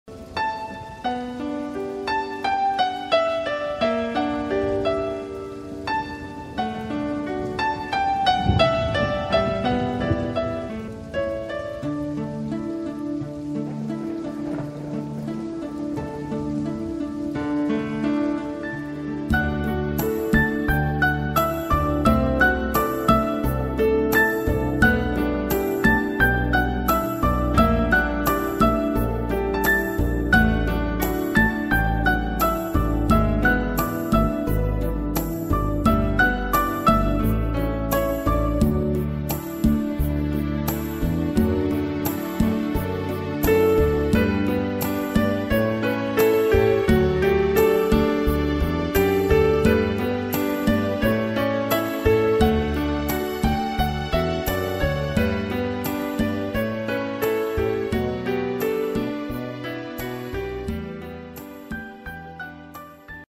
La Citta Penthouse condo sale 37 Mil. ,Sukhumvit - Thong Lo BTS.Bangkok
Property Code 13000946 -- Size 198 sqm. 3+1 Bedrooms.3+1 Bathrooms Sale 37,000,000 Baht